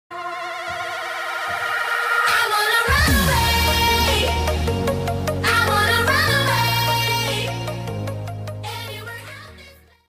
My new intro | Made by andrei skull gamer
The reason why I change my intro because there's so many intro like my intro in other youtuber that's why I change my intro

So I hope you enjoy my new intro